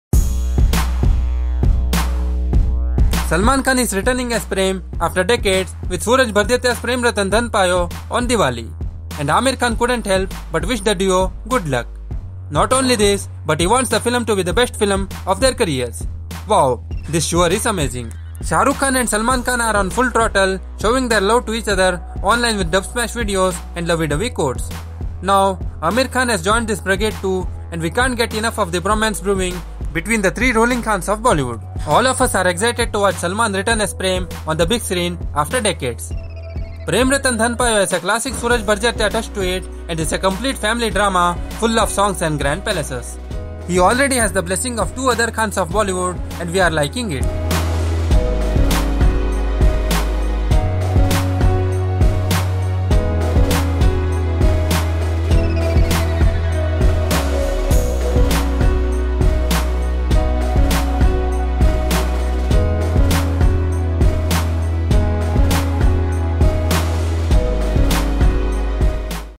Aamir Khan's Good Luck Message For Salman's Prem Ratan Dhan Payo !
Salman Khan is returning as Prem after decades with Sooraj Barjatya’s Prem Ratan Payo on Diwali and Aamir Khan couldn’t help but wish the duo good luck. Not only this, but he wants the film to be the best film of their careers! Wow this sure is amazing.